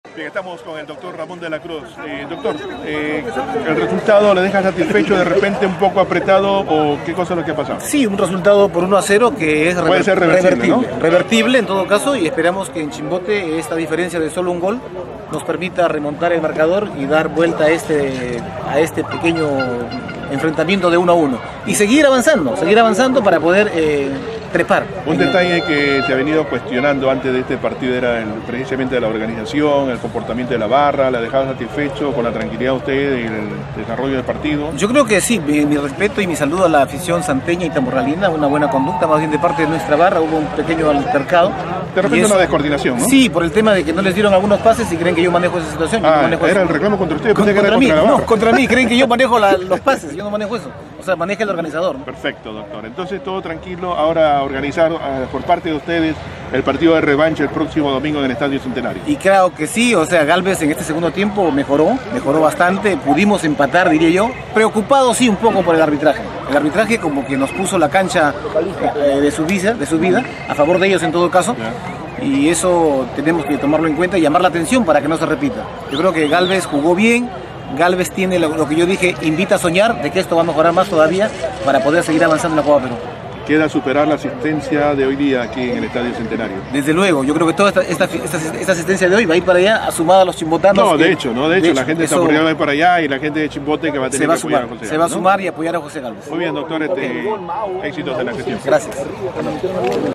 Juv. Tamborreal 2 José Gálvez 1 Copa Perú Dr. Ramón de la Cruz en Impacto Deportivo.
El Dr. Ramón de la Cruz, presidente del club José Gálvez de Chimbote; elogió el buen comportamiento del aficionado santeño en el estadio San Bartolo, luego que Juv. Tamborreal le ganara a José Gálvez 2-1 por la etapa departamental de la Copa Perú, hora espera el partido de vuelta el domingo 19 en el estadio Centenario Manuel Rivera Sánchez.